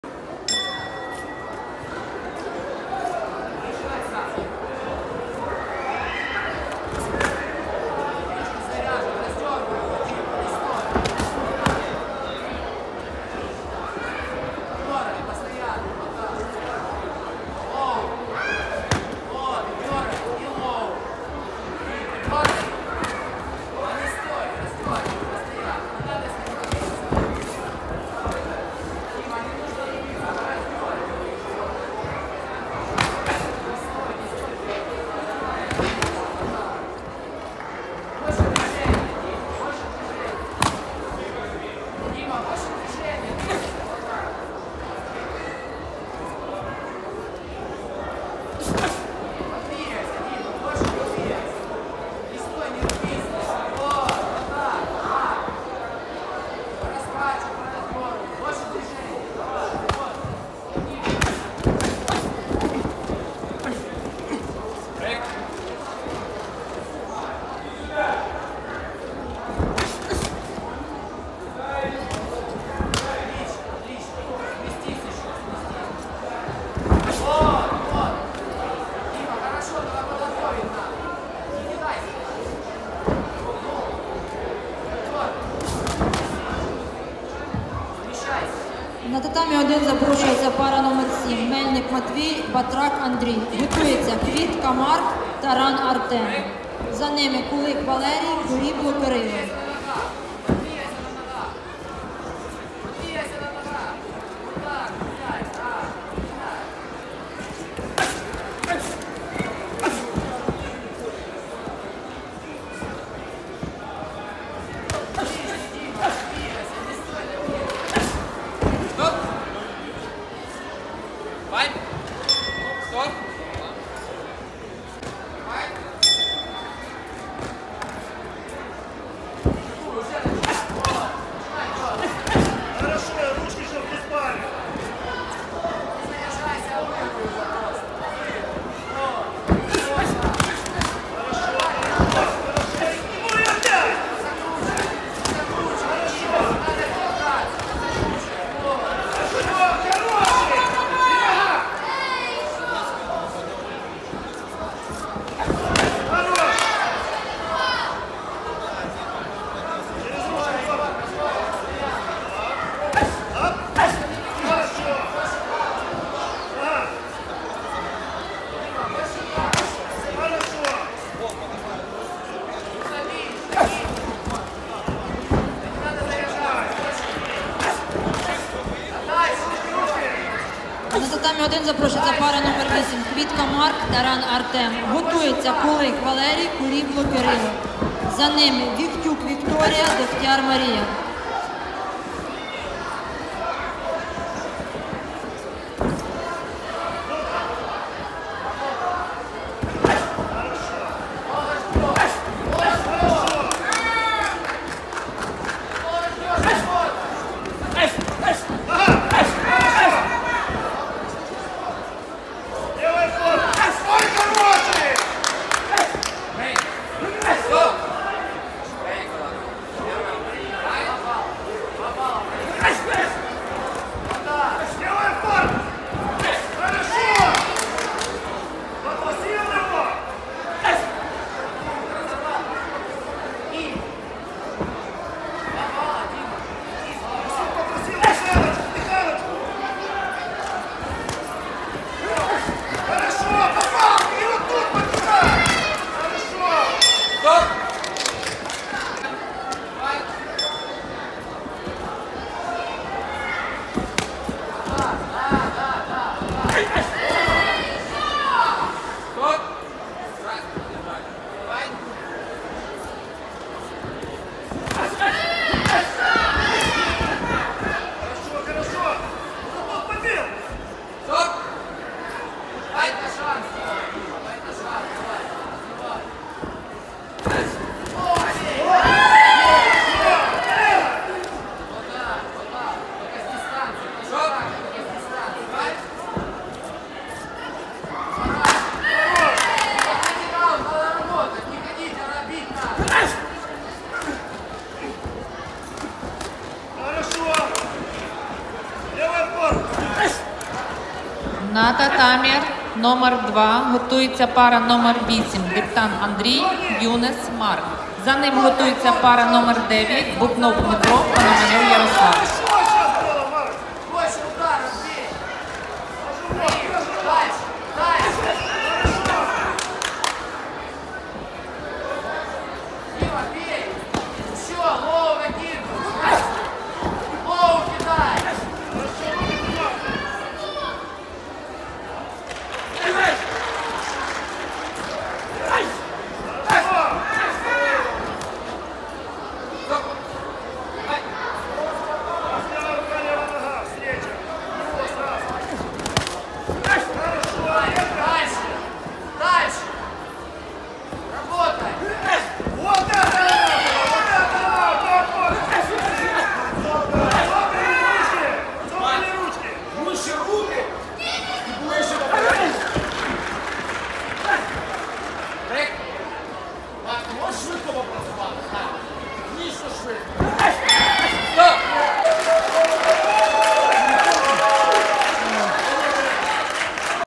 Кубок Києва з Кікбоксингу ВАКО 2023 К-1/Семенчук Максим(Кр) – Русецький Дмитро(Син) -63,5кг Фіннал
Кубок Києва з Кікбоксингу ВАКО 2023 К-1/ Семенчук Максим(Кр) – Русецький Дмитро(Син) -63,5кг Фіннал /Киів 2023г.
Semenchuk Maksym (Red)– Rusetskyi Dmytro (Blue) -63,5kg 1/2 / Cup of Kyiv WAKO К-1/Kyiv 2023.